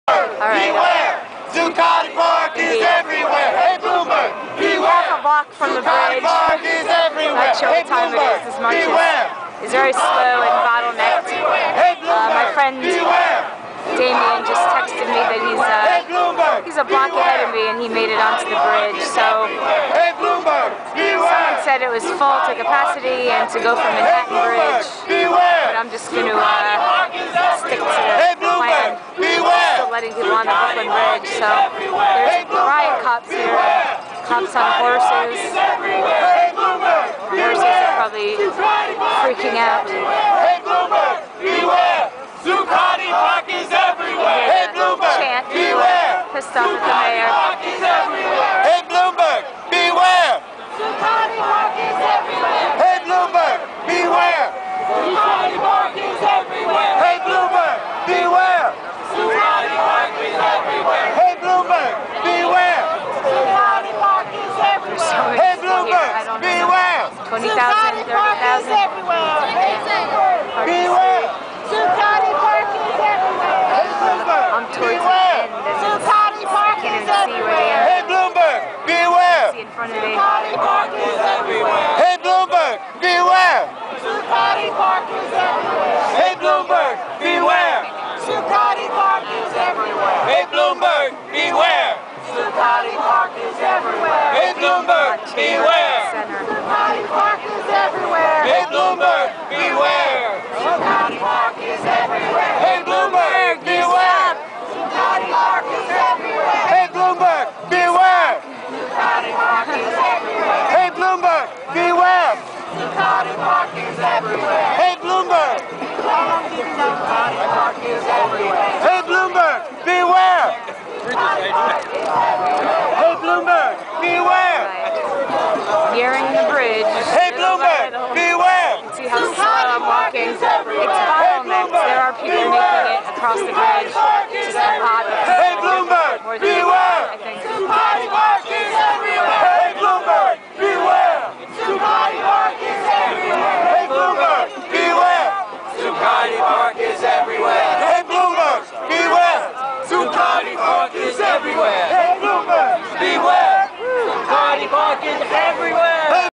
Approaching the brooklyn bridge..
Almost near the bridge on slow march from Foley..